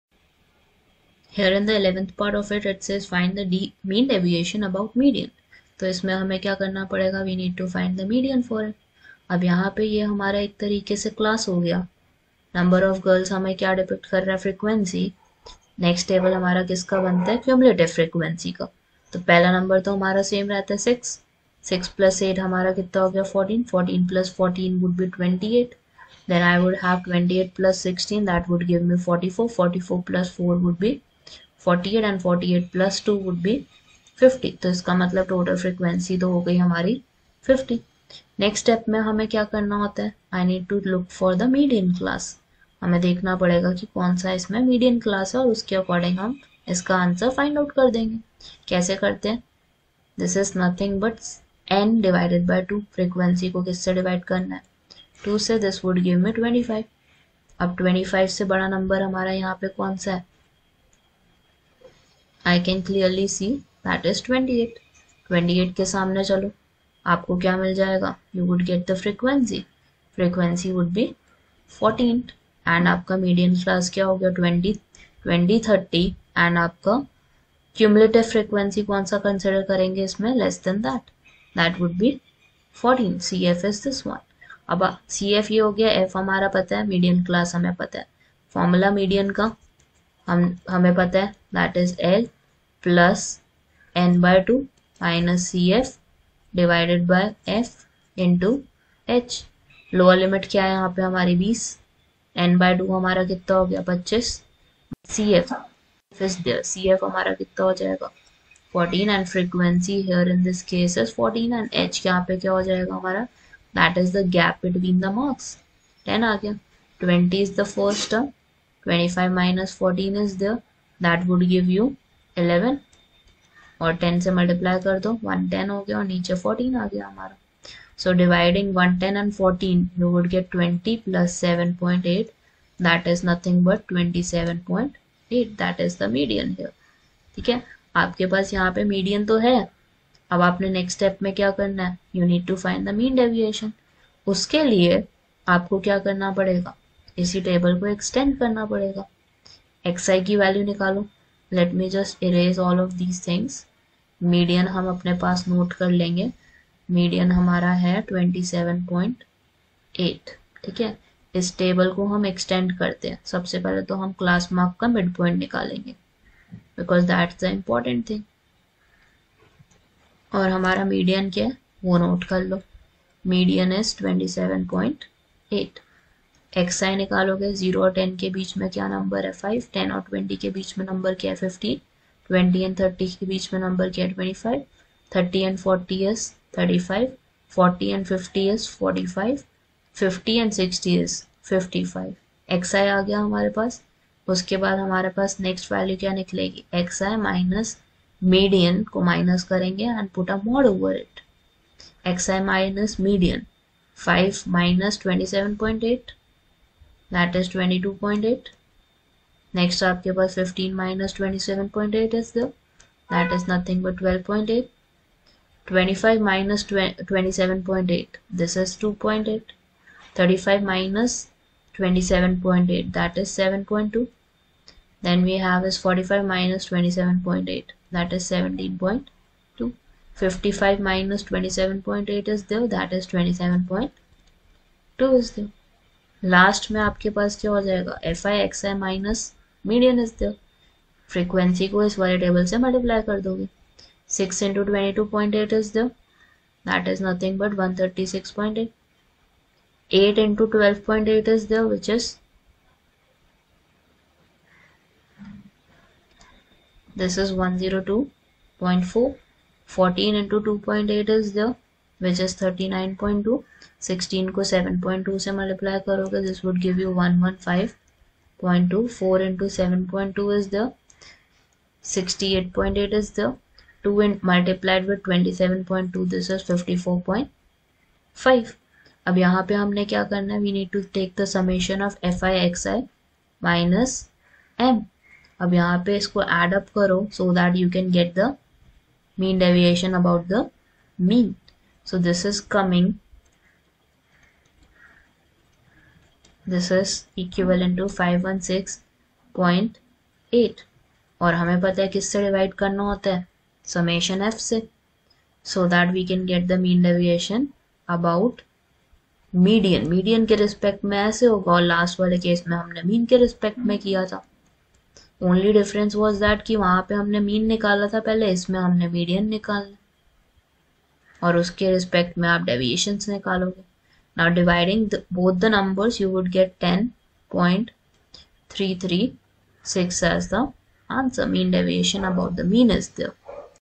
11. Find the mean deviation about median Marks 0-10 10-20 20-30 30-40 40-50 50-60 Number of Girls-
11. Find the mean deviation about median Marks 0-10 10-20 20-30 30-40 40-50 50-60 Number of Girls- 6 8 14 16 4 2